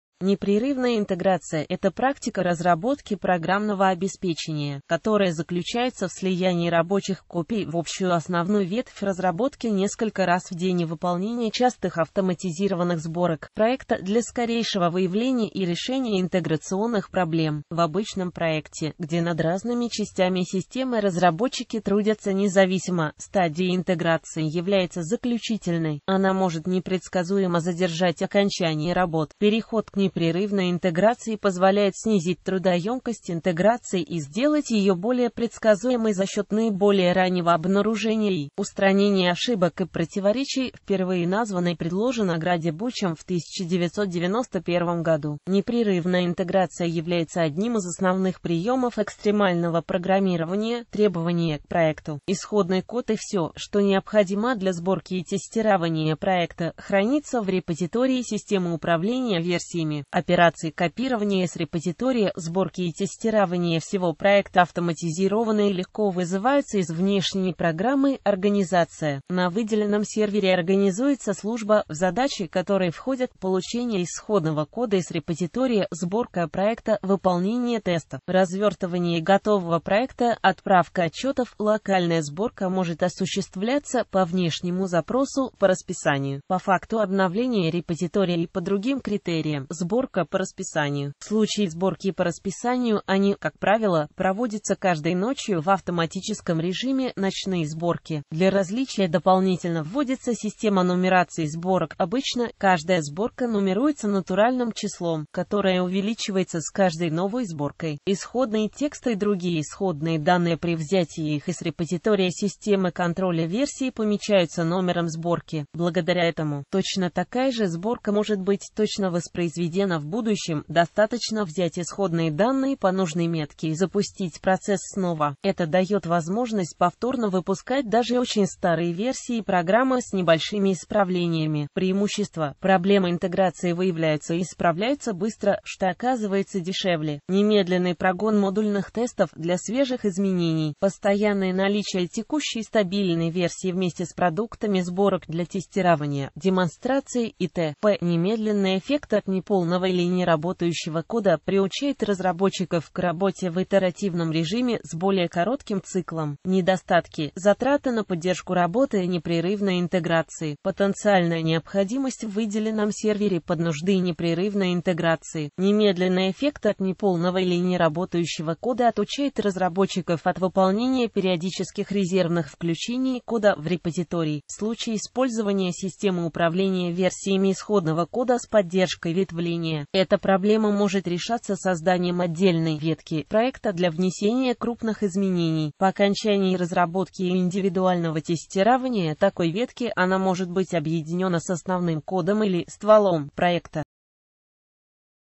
Непрерывная интеграция
Непрерывная интеграция — это практика разработки программного обеспечения, которая заключается в слиянии рабочих копий в общую основную ветвь разработки несколько раз в день и выполнении частых автоматизированных сборок проекта для скорейшего выявления и решения интеграционных проблем.В обычном проекте, где над разными частями системы разработчики трудятся независимо, стадия интеграции является заключительной.Она может непредсказуемо задержать окончание работ.Переход к непрерывной интеграции позволяет снизить трудоёмкость интеграции и сделать её более предсказуемой за счет наиболее раннего обнаружения и устранения ошибок и противоречий.